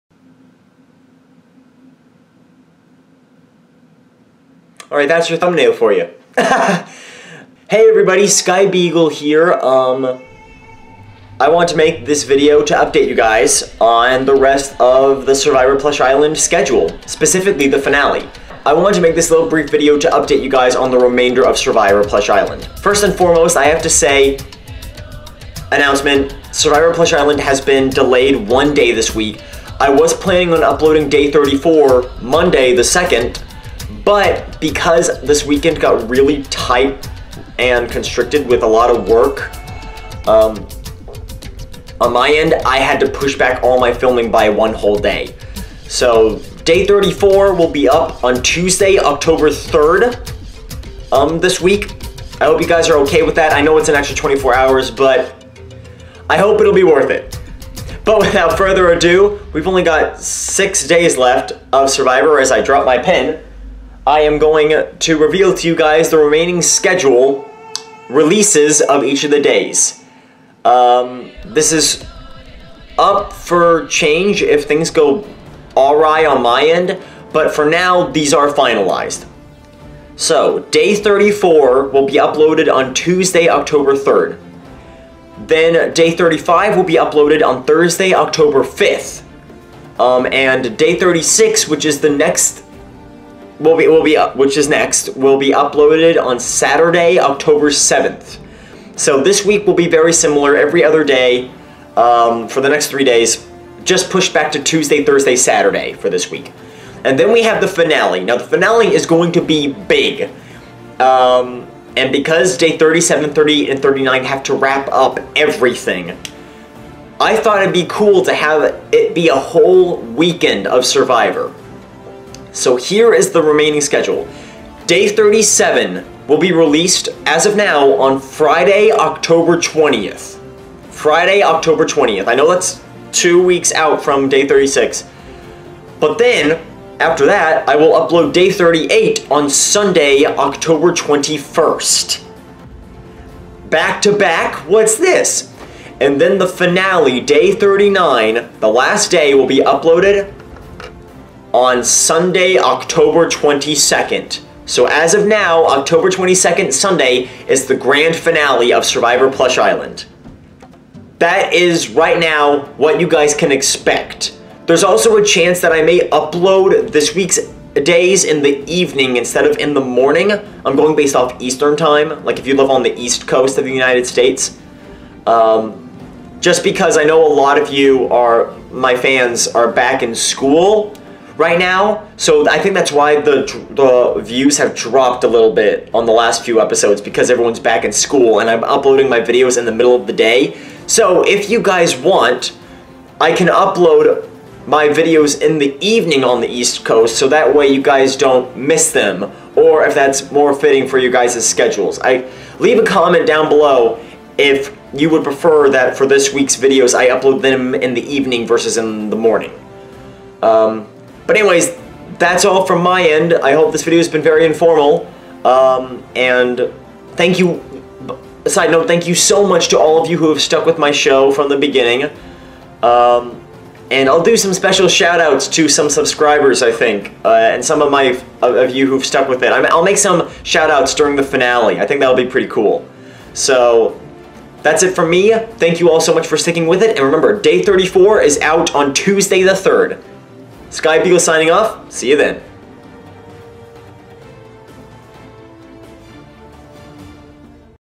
Survivor: Plush Island Finale Schedule RELEASED! (IMPORTANT)
Hey guys! Just a short video revealing the schedule for the remainder of Survivor: Plush Island!
Also, would you rather have me change my upload times from the morning to the evening (eastern time)? Leave your choice in the comments.
Thanks for sticking with this show, guys! It means a lot to me, and I hope you guys enjoy the remaining few days left!